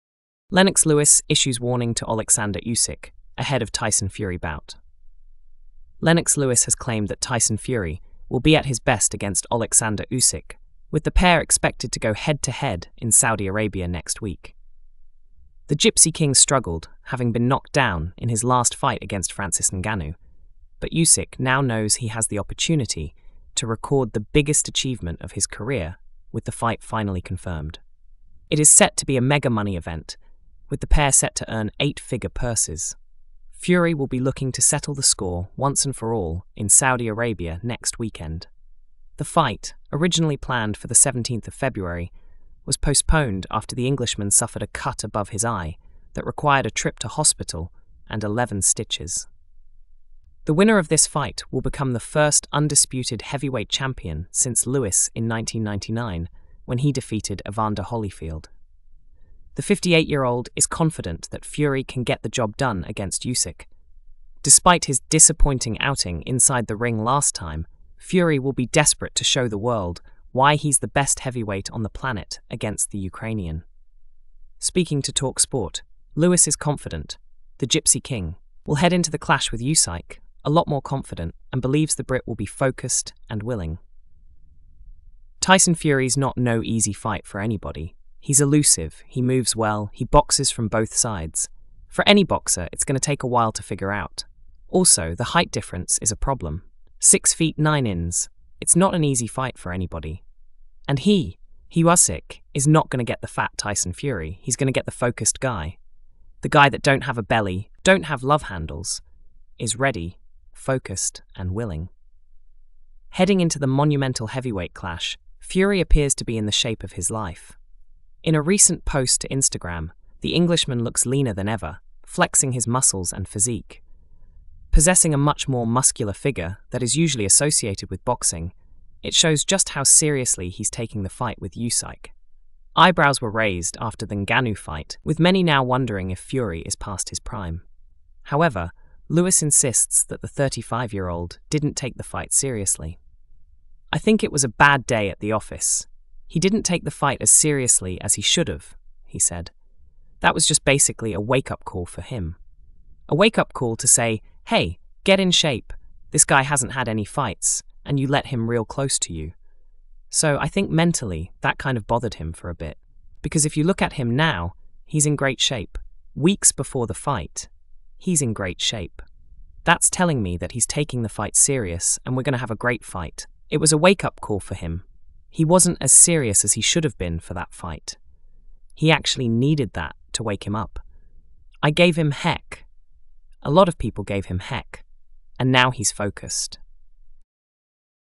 Lennox Lewis Issues Warning to Oleksandr Usyk Ahead of Tyson Fury Bout
Description: Lennox Lewis sends a cautionary message to Oleksandr Usyk in anticipation of his upcoming bout against Tyson Fury. Delve into Lewis's insights as he sheds light on the challenges Usyk may face in the ring.